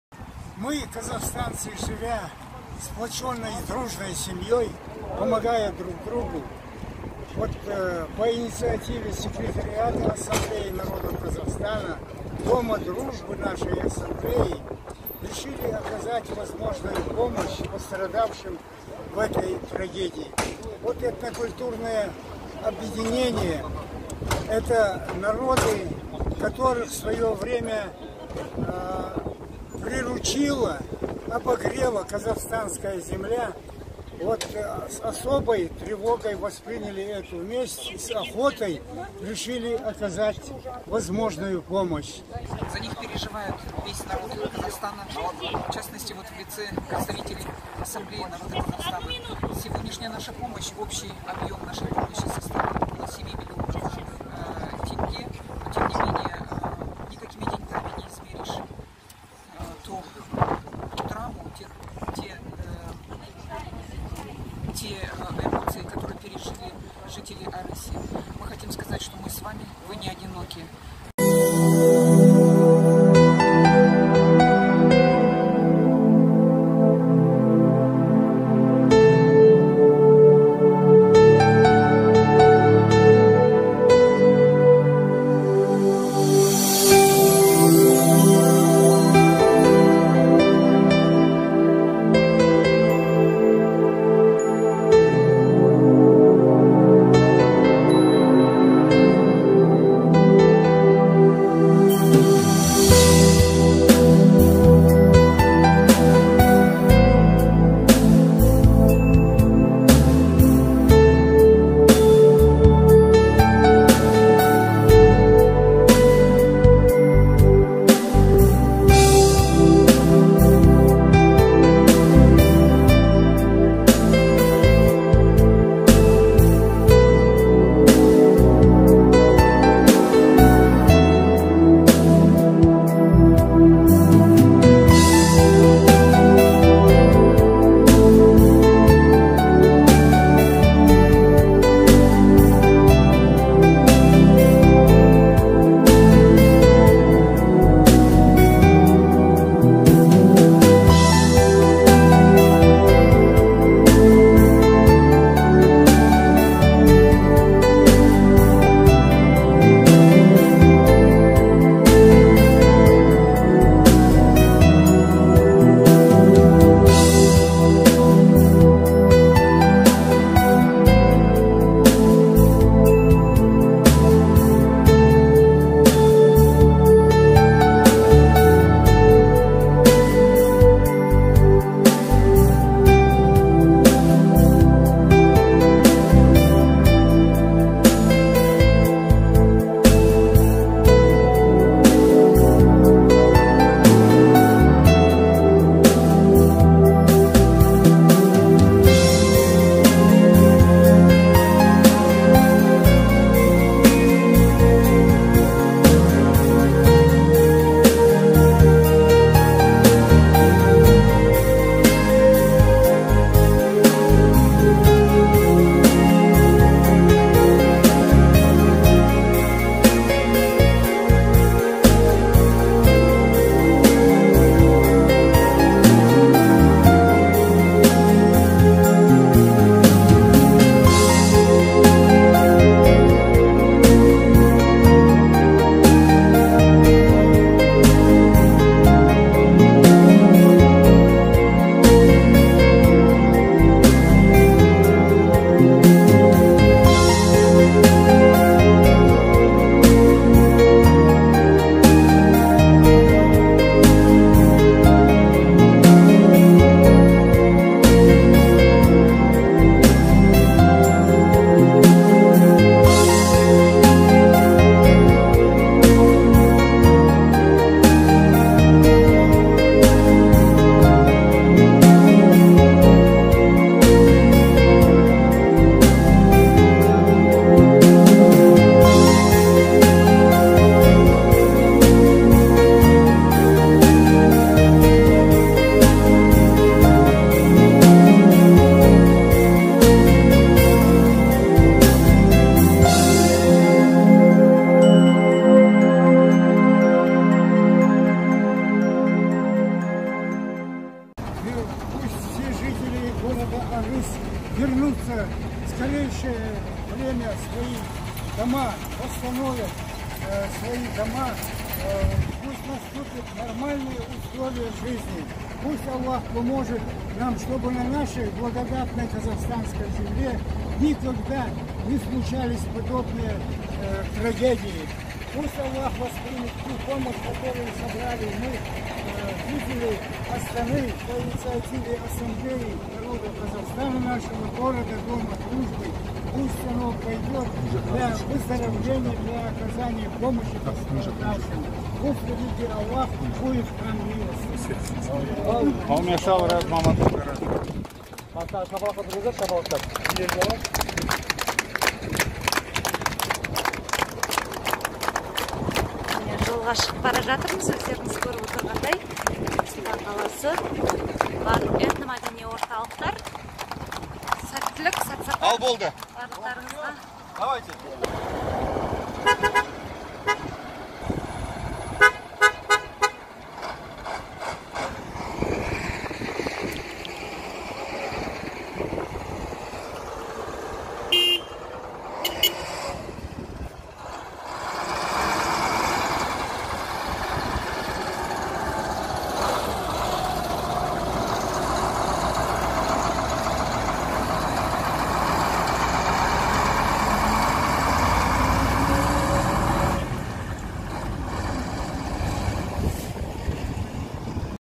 Нұр-Сұлтан қаласы, "Арыс, біз біргеміз!" акциясы
Нұр-Сұлтан қаласы Достық үйінде арнайы құрылған штаб "Арыс, біз біргеміз!" акциясына байланысты жиналған азық-түлік, тұрмыстық заттар және киім-кешектер мен төсек-орындарды Түркістан облысының 40 тонналық арнайы жүк көлігіне тиеп жөнелтті